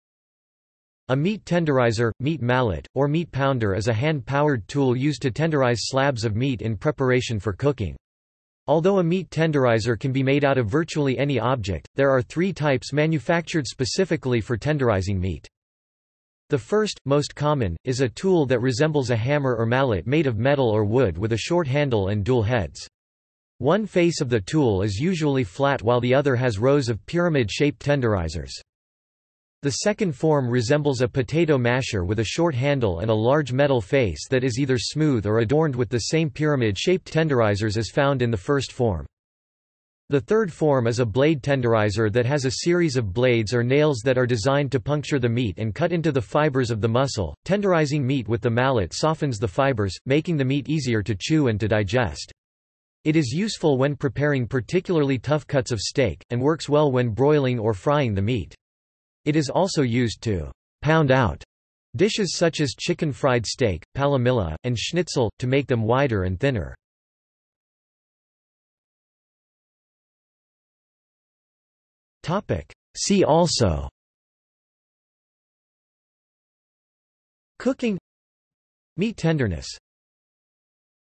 Meat tenderizer | Wikipedia audio article
This is an audio version of the Wikipedia Article:
Meat tenderizer

00:01:08 undefined

- Socrates

SUMMARY
A meat tenderizer, meat mallet, or meat pounder is a hand-powered tool used to tenderize slabs of meat in preparation for cooking. Although a meat tenderizer can be made out of virtually any object, there are three types manufactured specifically for tenderizing meat.

The first, most common, is a tool that resembles a hammer or mallet made of metal or wood with a short handle and dual heads. One face of the tool is usually flat while the other has rows of pyramid-shaped tenderizers.
The second form resembles a potato masher with a short handle and a large metal face that is either smooth or adorned with the same pyramid-shaped tenderizers as found in the first form.
The third form is a blade tenderizer that has a series of blades or nails that are designed to puncture the meat and cut into the fibers of the muscle.Tenderizing meat with the mallet softens the fibers, making the meat easier to chew and to digest.  It is useful when preparing particularly tough cuts of steak, and works well when broiling or frying the meat.  It is also used to "pound out" dishes such as chicken-fried steak, palomilla, and schnitzel, to make them wider and thinner.